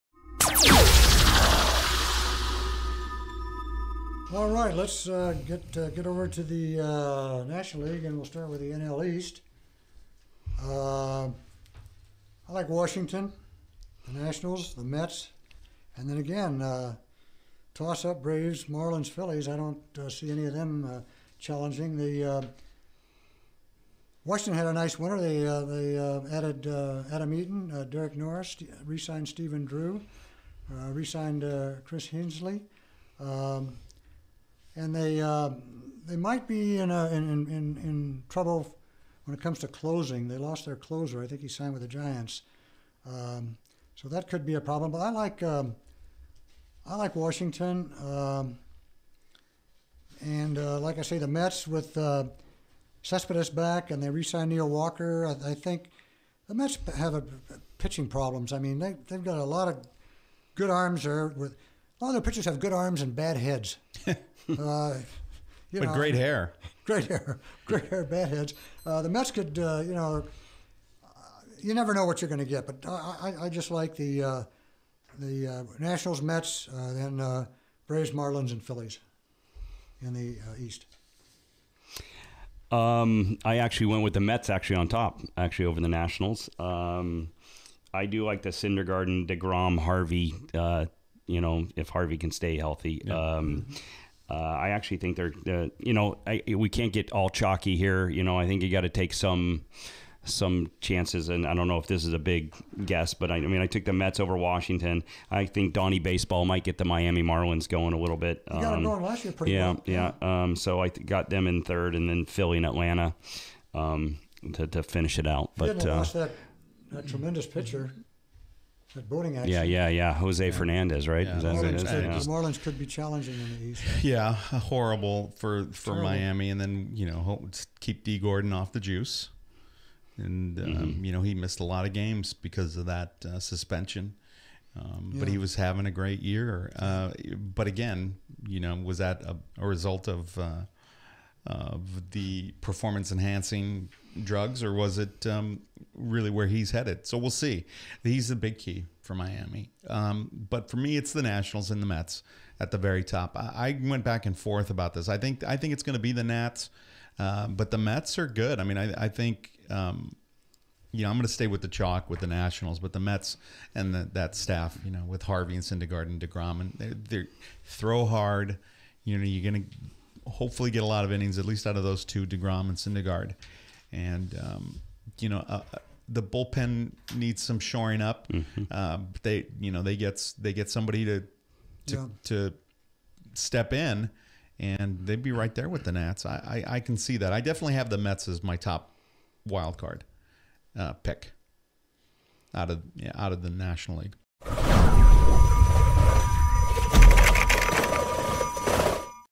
National League East Predictions - Will The Mets Win It All?
Will the New York Mets win the whole National League East division in Major League Baseball?

There are lots of potential baseball teams that could not just win the National League East but also the pennant. John Stellman, Vince Morales and John Adams all seem to think the Mets are the team to beat coming out of the National League East in MLB.

The question is can the Mets win the pennant this year?